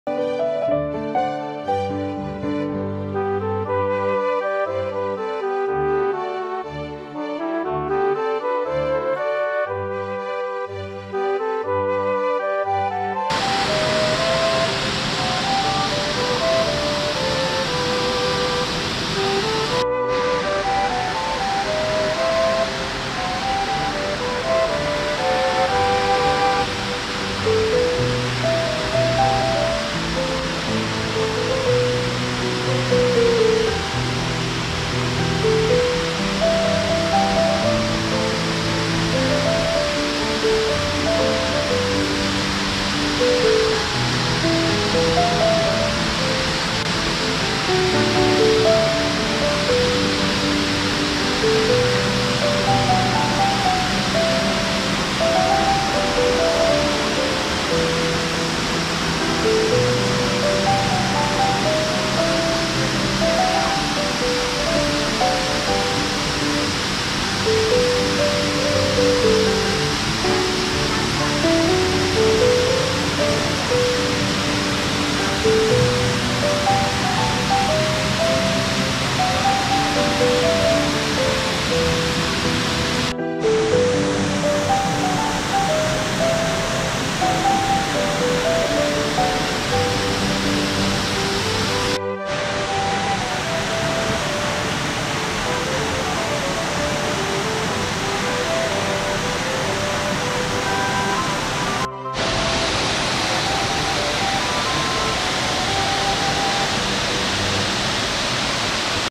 Travel around Oregon - Silver Falls State Park - Lower South Falls
2020 Fall travel hiking at Silver Falls State Park Oregon, hiking at lower south falls